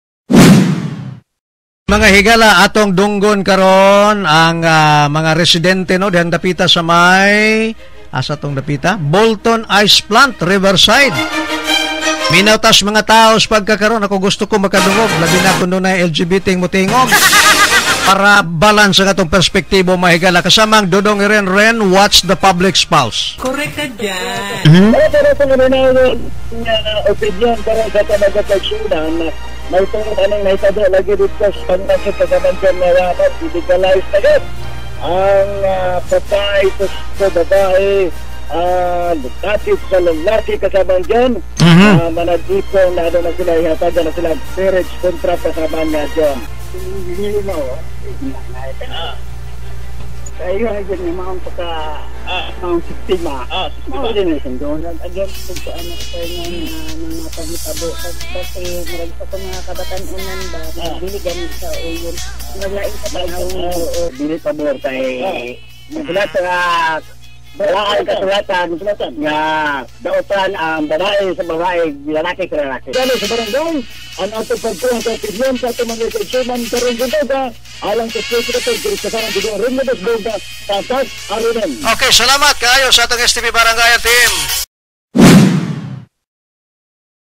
STP SA BARANGAY 1-A BOLTON ICE PLANT
STRAIGHT TO THE POINT SA BARANGAY KAUBAN SI KASAMANG JUNE DUTERTE UG KASAMANG ERIN LUMOSBOG SA HISOTANAN NGA 'SAME-SEX MARRIAGE' ALANG SA ADLAW NGA LUNES JUNE 29, 2015

Listen to DXDC @ 621 khz sa AM radio dial and via streaming @